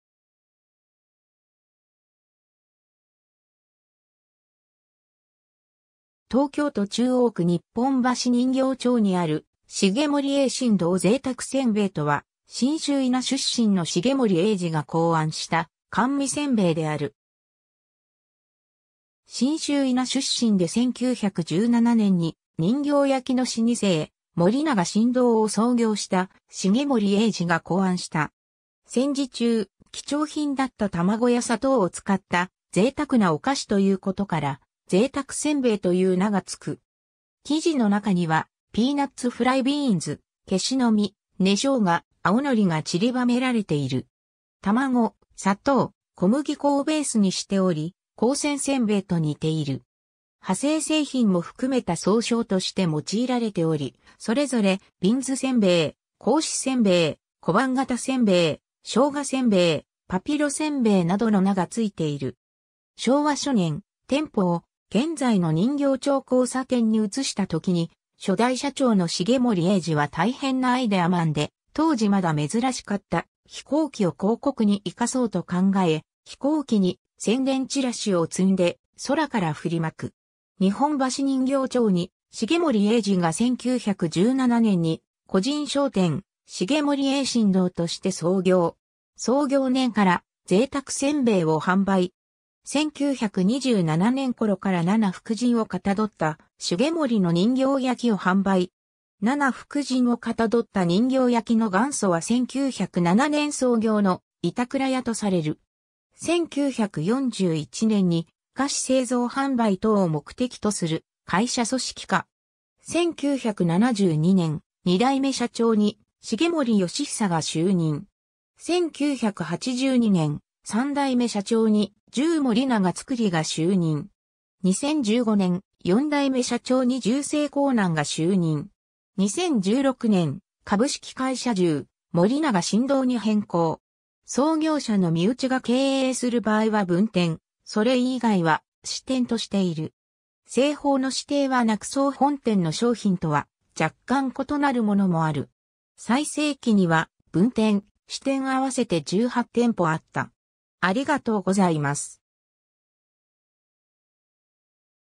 ゼイタク煎餅
東京都中央区日本橋人形町にある重盛永信堂 ゼイタク煎餅（ゼイタクせんべい）とは、信州伊那出身の重盛永治が考案した甘味煎餅である。
 信州伊那出身で1917年に人形焼の老舗重盛永信堂を創業した重盛永治（1890年 - ？）が考案した。
 戦時中、貴重品だった卵や砂糖を使った「贅沢な」お菓子ということからゼイタク煎餅という名がつく。
 生地の中にはピーナッツ・フライビーンズ・けしの実・根生姜・青のりがちりばめられている。
 卵・砂糖・小麦粉をベースにしており、鉱泉煎餅と似ている。
 派生製品も含めた総称として用いられており、それぞれ「ビンズ煎餅」「格子煎餅」「小判形煎餅」「生姜煎餅」「パピロ煎餅」などの名がついている。
 昭和初年、店舗を現在の人形町交差点に移したときに初代社長の重盛永治は大変なアイデアマンで、当時まだ珍しかった飛行機を広告に活かそうと考え、飛行機に宣伝チラシを積んで空からふりまく（日本初のフライヤー広告）。
 日本橋人形町に重盛永治が1917年に個人商店「 重盛永信堂」として創業。
創業年からゼイタク煎餅を販売。
1927年頃から七福神を型どった「重盛の人形焼」を販売。
※七福神を型どった人形焼の元祖は1907年創業の板倉屋とされる。
 1941年に菓子製造販売等を目的とする会社組織化。
 1972年、2代目社長に重盛好久が就任。
 1982年、3代目社長に重盛永造が就任。
 2015年、4代目社長に重盛行男が就任。
 2016年、株式会社重盛永信堂に変更。
 創業者の身内が経営する場合は分店、それ以外（甥を含む）は支店としている。
製法の指定はなく総本店の商品とは若干異なるものもある。
最盛期には分店・支店合わせて18店舗あった。